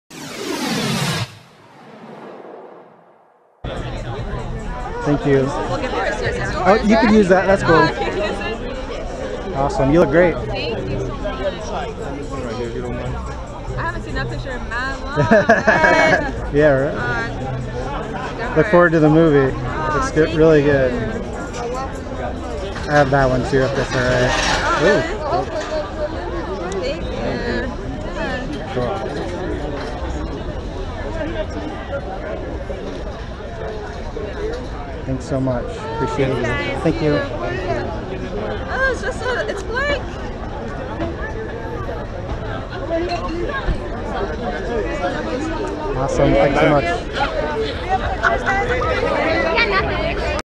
Gracie Marie Bradley signs autographs for TopPix
in New York City.

For authentic autographed memorabilia of Gracie Marie Bradley and many other celebrities, check out TopPix Autographs